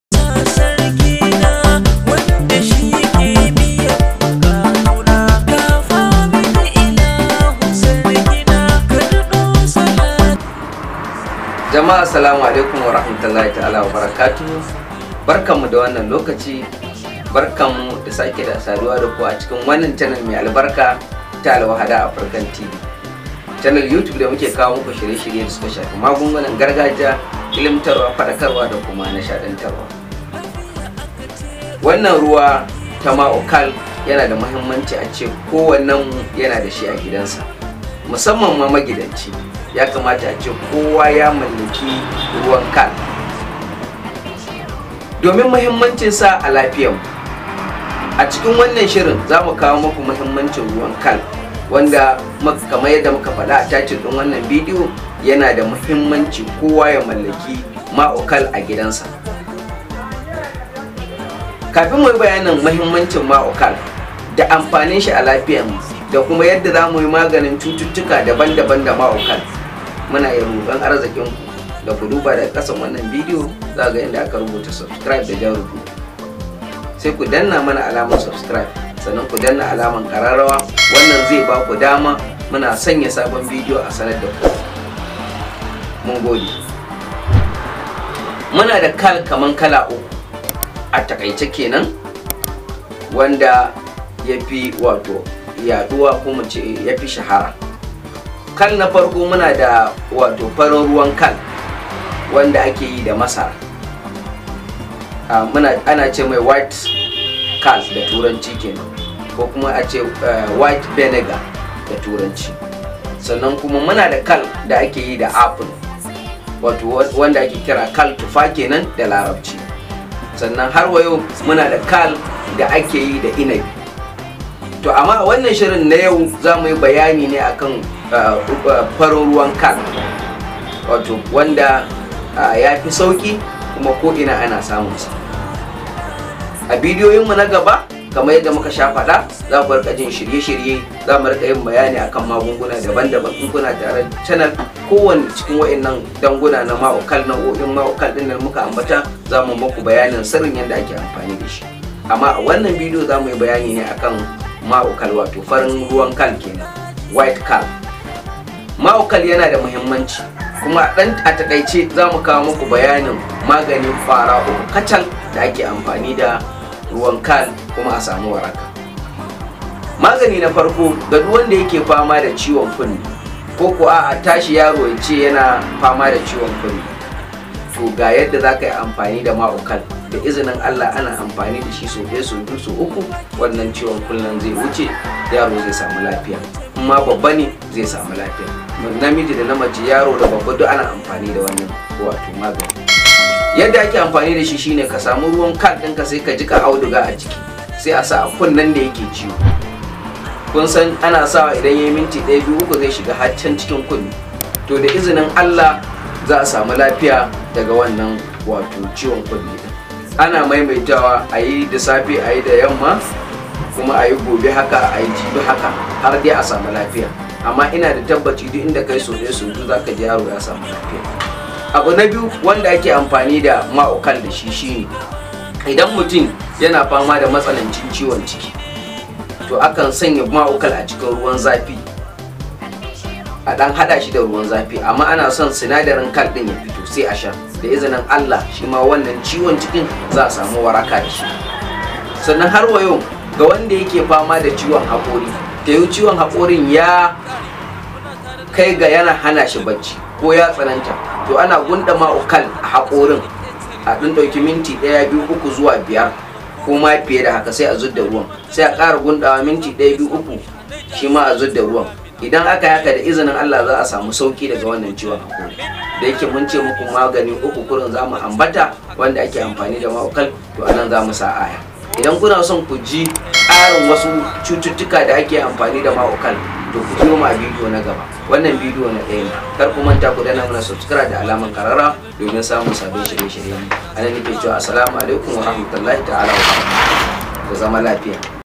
Yana da muhimmanci duk wani magidanci ya kasance a kodayaushe baya rasa Ma'ul-Khal a gida.
Barkan mu da warhaka!